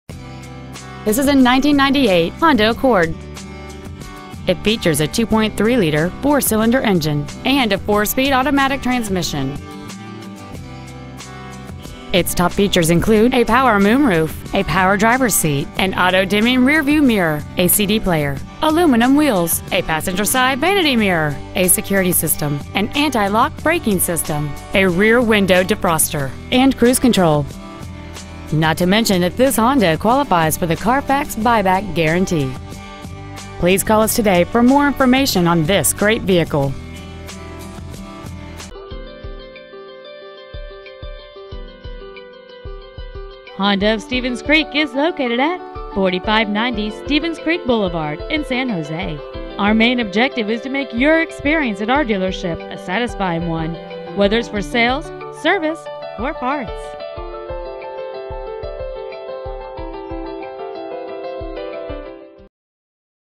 1998 Honda Accord San Jose CA 95129
Year: 1998
 Make: Honda
 Model: Accord
 Engine: 2.3L I-4 cyl
 Trans.: 4-Speed Automatic
 Exterior: Gold
 Miles: 109,236
 Interior: Beige

 Used 1998 Honda Accord San Jose CA